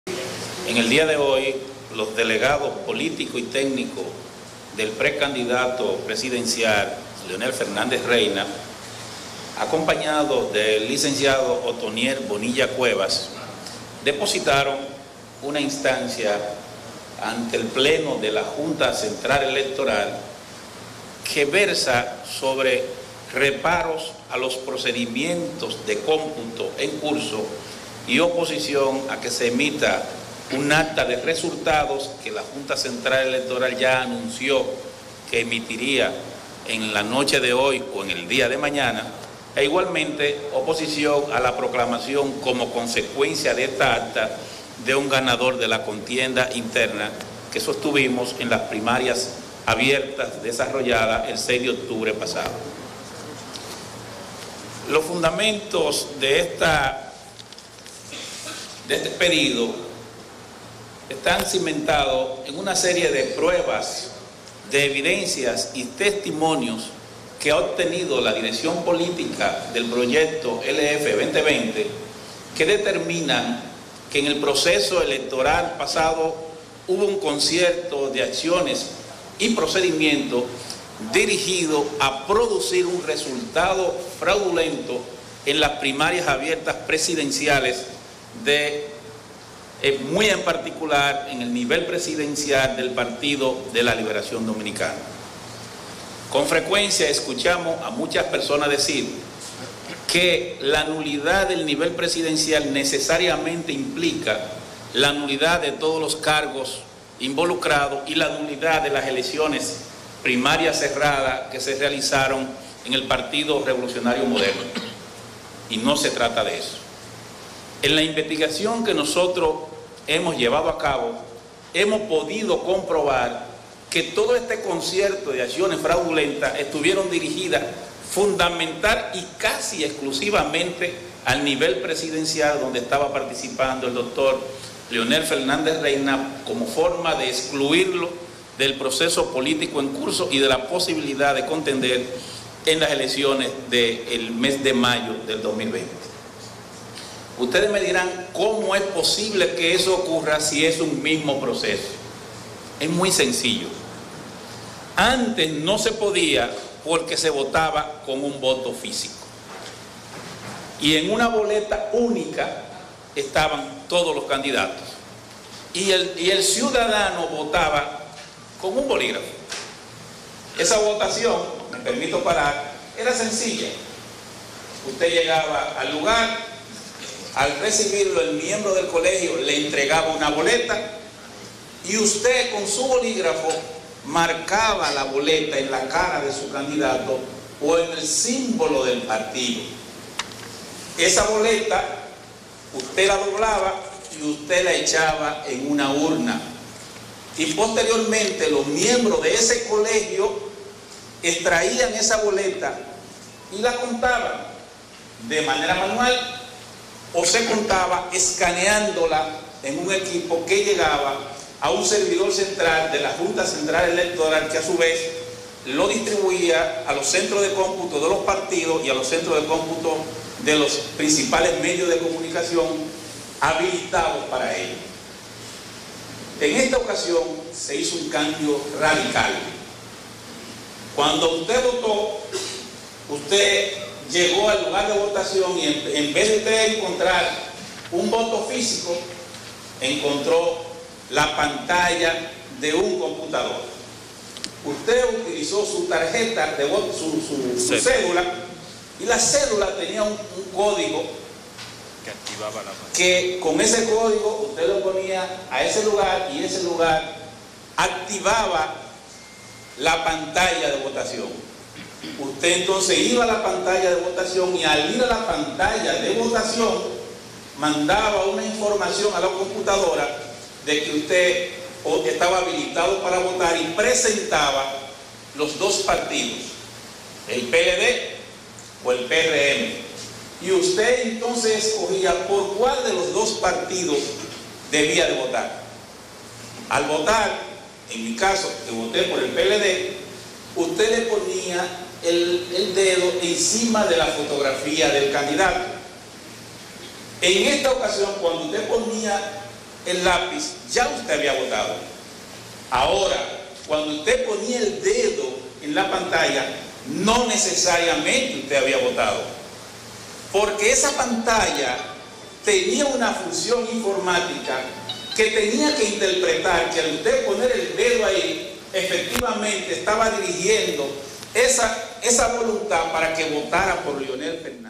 Equipo de Leonel Fernández habla sobre supuesto fraude en las primarias del seis de octubre
Santo Domingo, RD.- El equipo político de Leonel Fernández habla sobre supuesto fraude en las primarias del pasado seis de octubre.